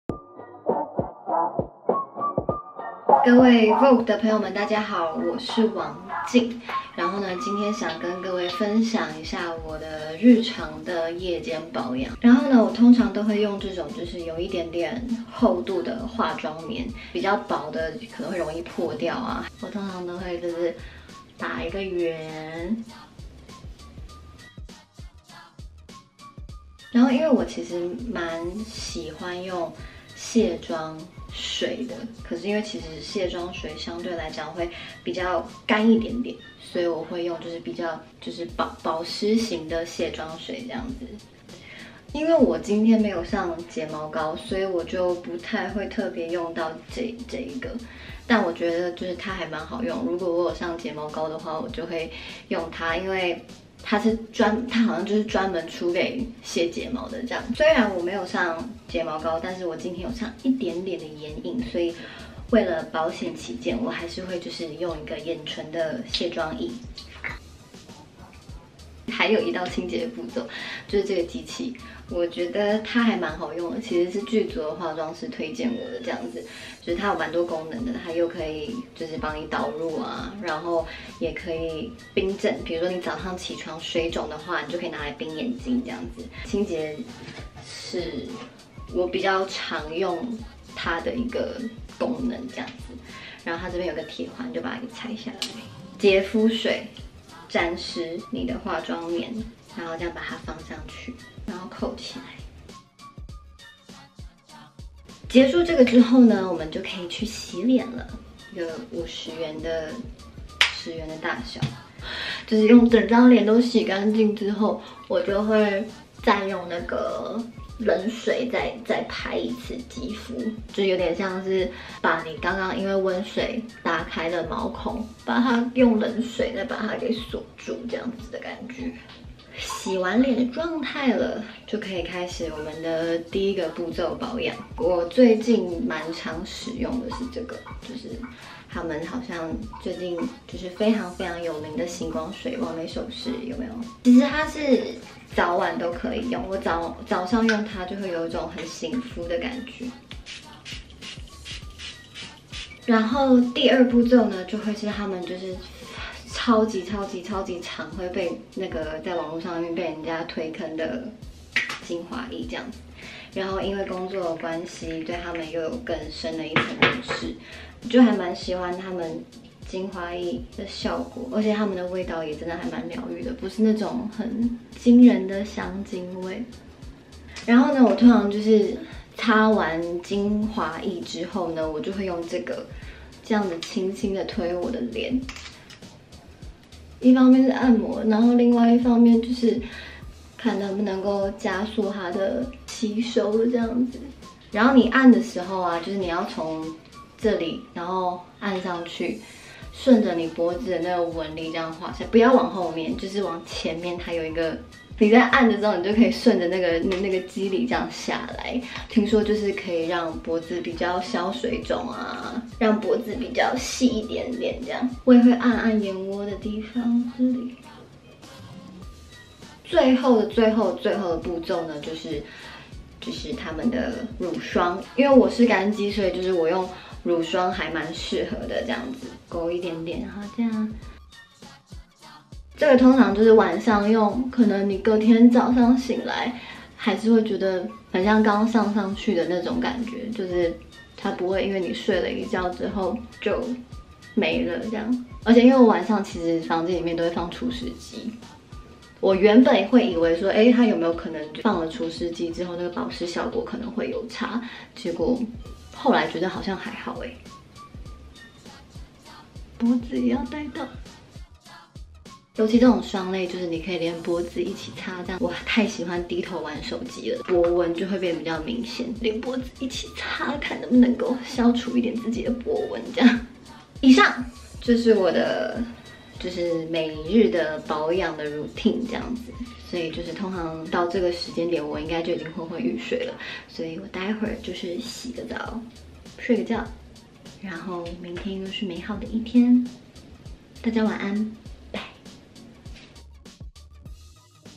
養出無瑕肌膚！王淨的睡前保養步驟大公開｜VOGUE Girl來開箱
想要肌膚無瑕淨透，保養清潔絕對是關鍵！本周Vogue Girl請來女星王淨來為大家示範她的日常保養步驟，從卸妝洗臉到最後一層保養，有很多小秘訣值得學習唷！